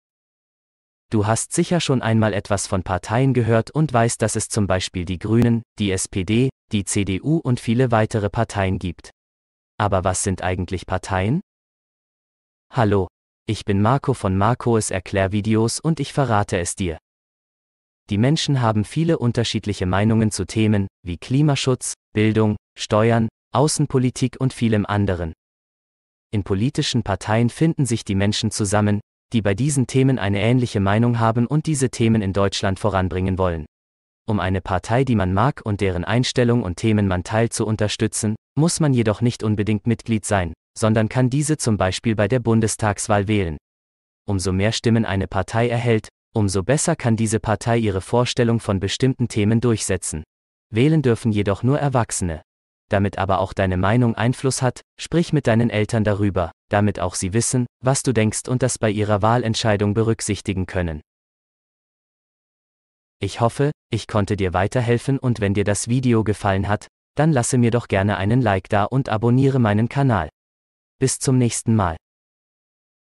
Was sind Parteien? - Lern- und Erklärvideo für Grundschul-Kinder.
Warum organisieren sich Menschen in politischen Parteien? Die Antwort findest du in diesem Video.